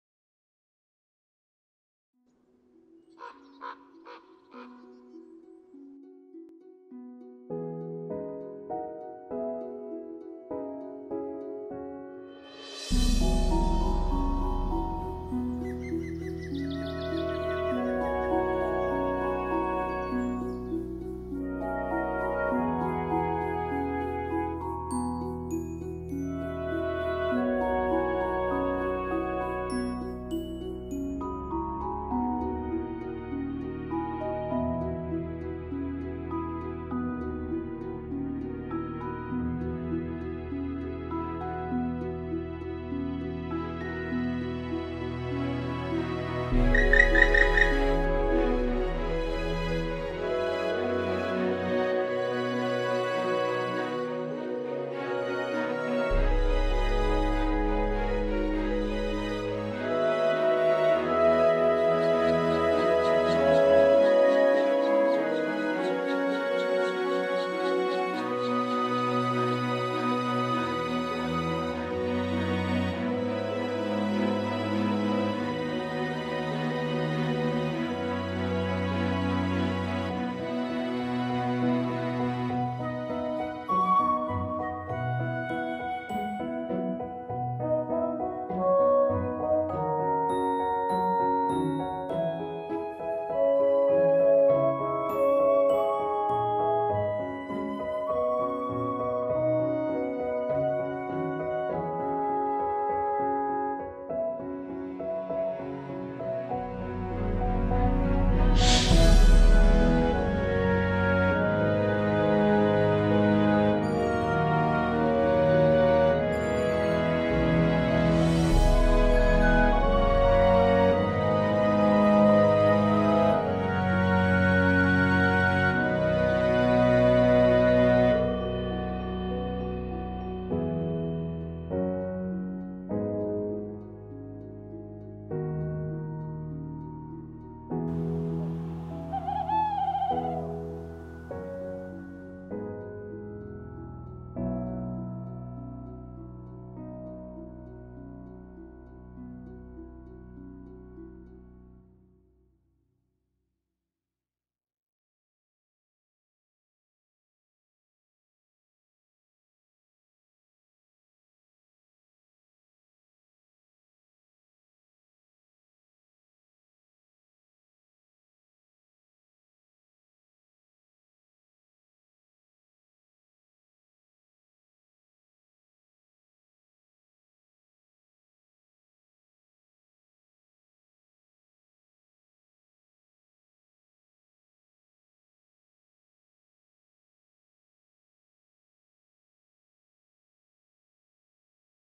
Naturfilm Hüttener Berge 2020
Der Naturpark Hüttener Berge liegt direkt vor der Haustür des Ostseebades Eckernförde. Zahlreiche Wälder, Seen, Tierarten und eine sehr abwechslungsreiche Natur erwarten Dich hier. Schau doch mal rein